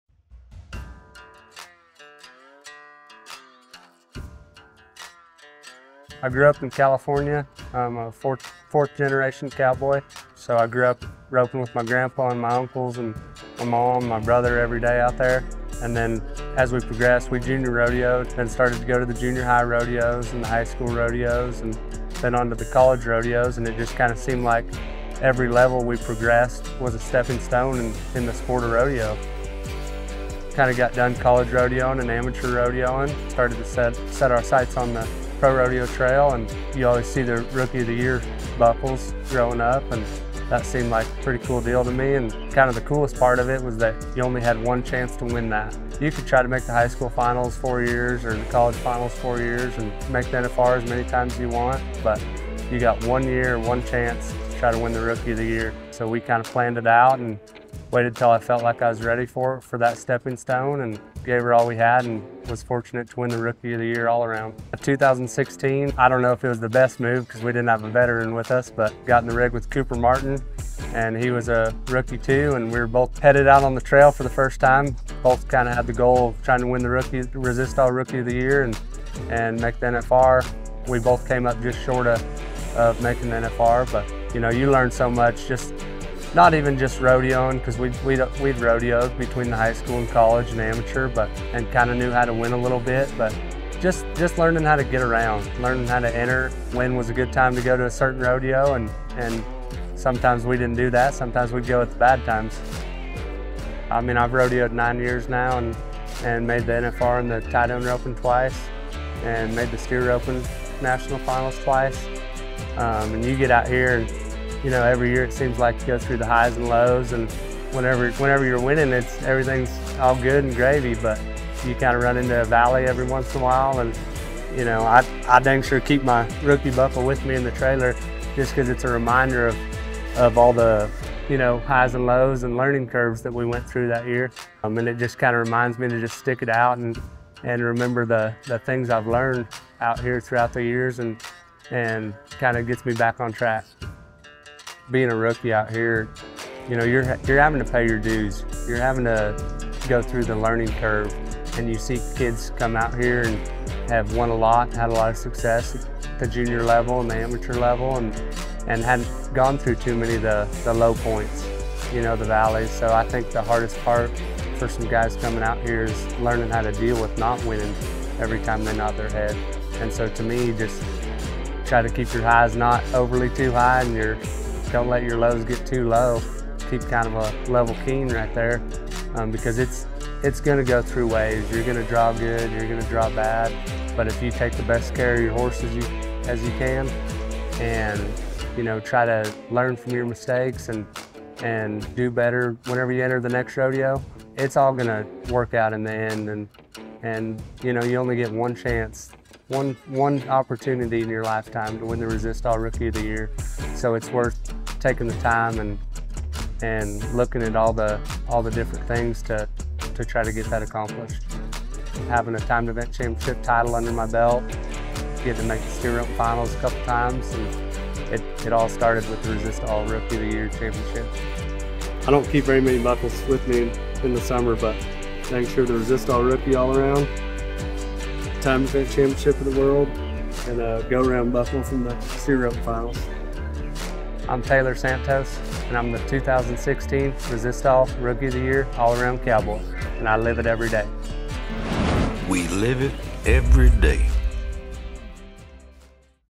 Resistol Rookie Rewind - Taylor Santos
All Around Cowboy Taylor Santos comes from a legendary rodeo family and always had the Resistol Rookie of The Year title in his sights. There's basically nothing this guy can't do with a rope in his hand. He is 100% Cowboy and winning the Rookie of the Year was the start of a career that lead him to multiple National Finals Rodeo qualifications.